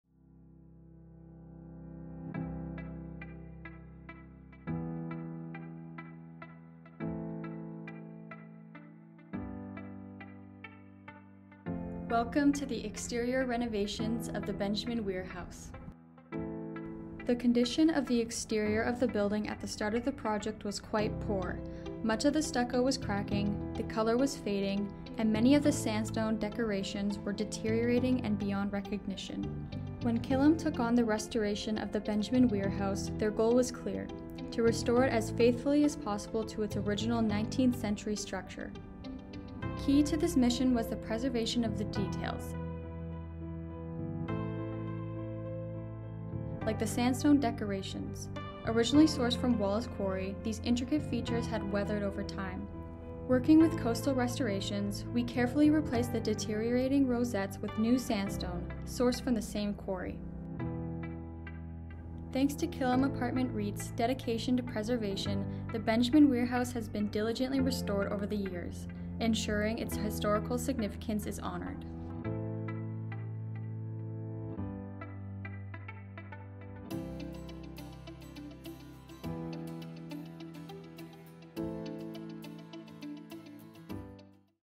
Exterior Renovations - Standstone
Every sandstone block in the Benjamin Wier House carries the weight of time.

Through the tireless efforts of our team, in collaboration with skilled stone masons and consultants, this historic treasure has been thoughtfully rejuvenated. Their careful craftsmanship ensures that this landmark remains a testament to Halifax's enduring legacy, harmonizing the old with the new.

This restoration reflects the dedication and expertise of all involved, revitalizing a space that continues to influence the evolving narrative of our city.